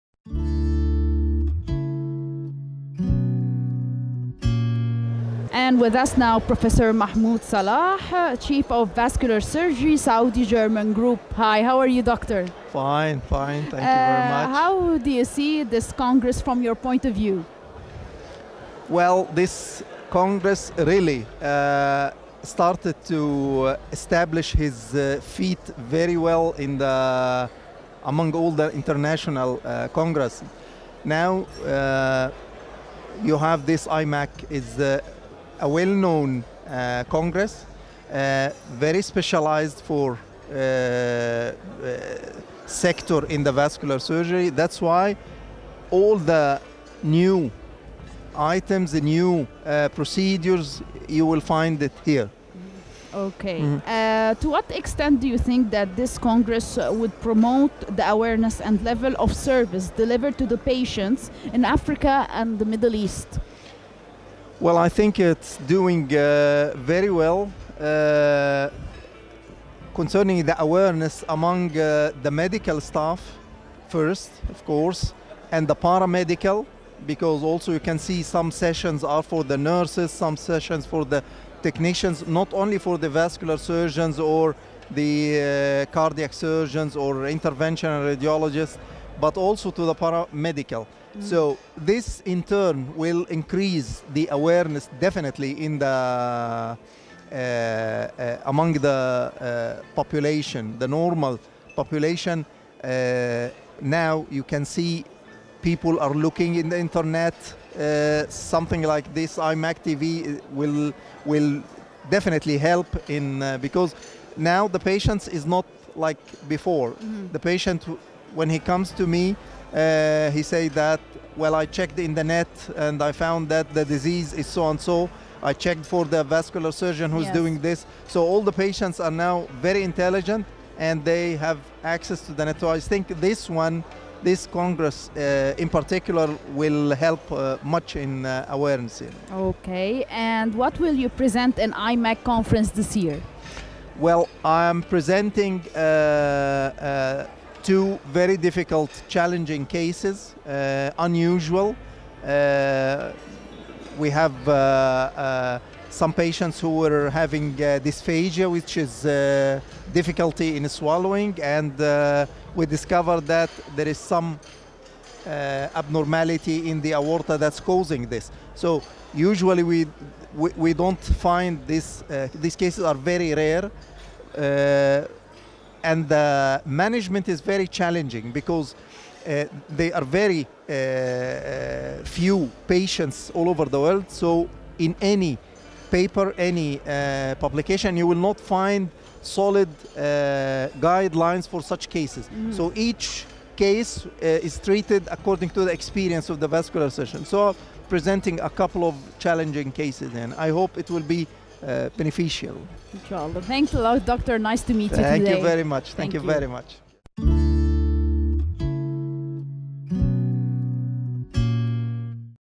Prof.Mahmoud Salah talks about IMAC conference.
Prof  Mahmoud Salah
Chief of Vascular Surgery Saudi German Group
الاستاذ الدكتور/ محمود صلاح
 رئيس جراحة الأوعية الدموية بالمجموعة السعودية الألمانية